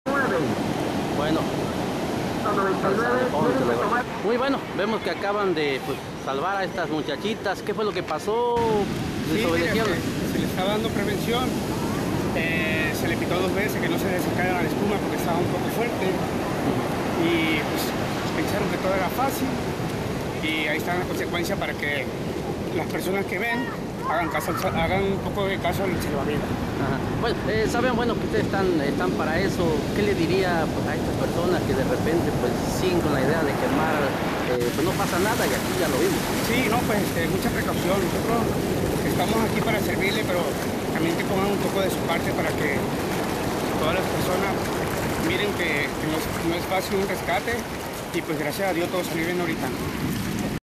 RESCATAN JOVENES TURISTAS EN IXTAPA
Rescate de dos jóvenes turistas en Ixtapa, no acataron las recomendaciones y el mar estuvo a punto de llevárselas durante las marejadas de los últimos días de semana santa.